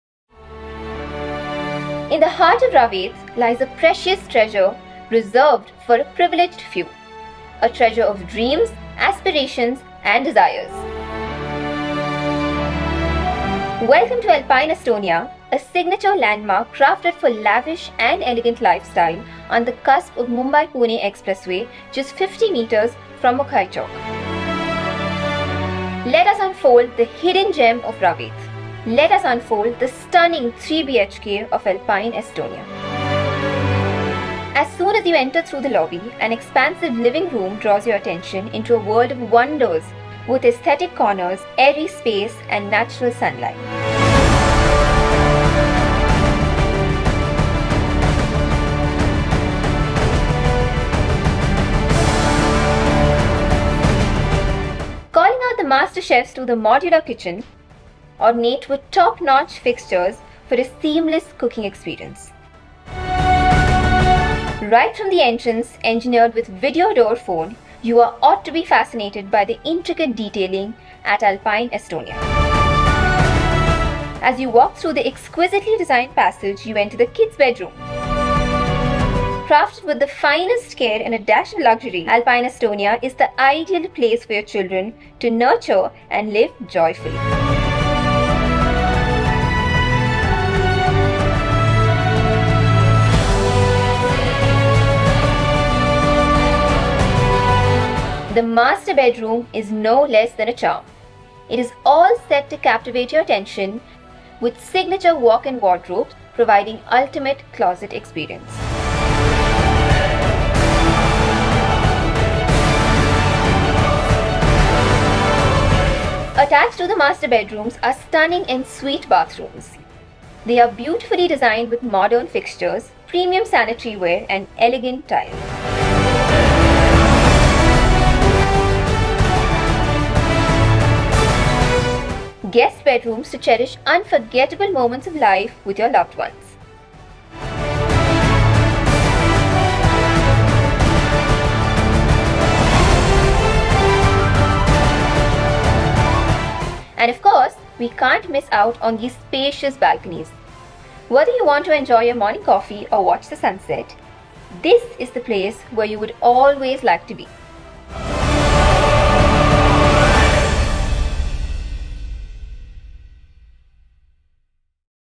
Sample Flat Virtual Tour | Exclusive 3 BHKs at Alpine Astonia | Ravet
Unfold the grand 3 BHKs at Alpine Astonia with Avani Awasthee, a lifestyle & fashion influencer.

Discover the exclusive residences, designed with precision and perfection in Ravet, just 50m from Mukai Chowk.

- Lavish 3 BHKs with Walk-in Wardrobes
- Video Door Phone at Entrance
- Premium Fittings & Specifications
- Chosen by 100+ Families

Schedule site visit today!